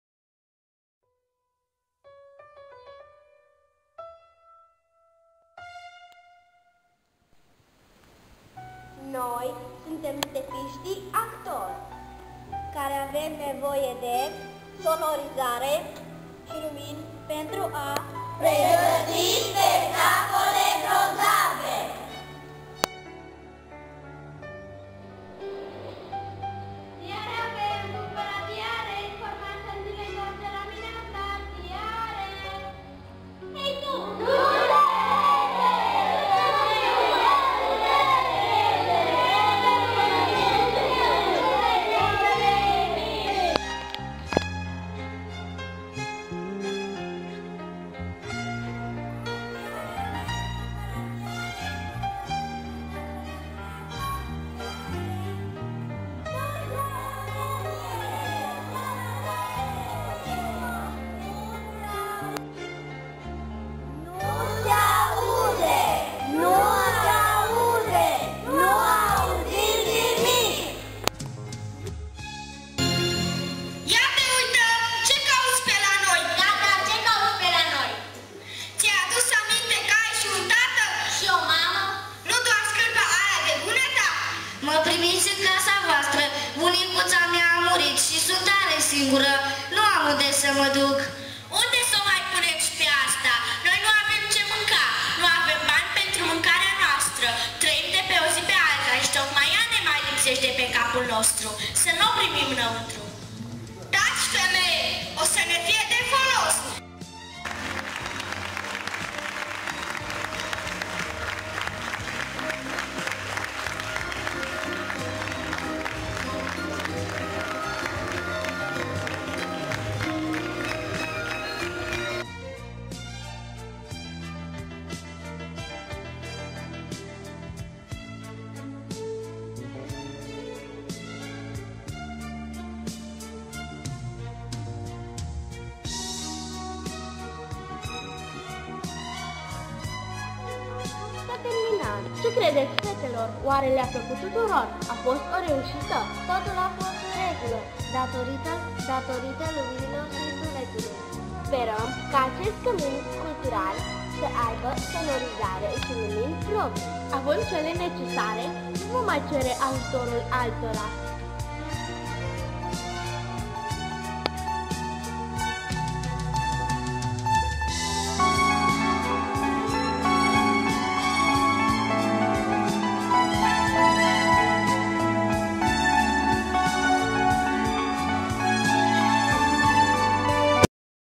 Stepistii actori
Prin piesele de teatru pe care le interpretam cu diferite ocazii pe scena amenajata,  noi, Stepistii Actori dorim sa relansam interesul satenilor pentru cultura, mai ales pentru teatru.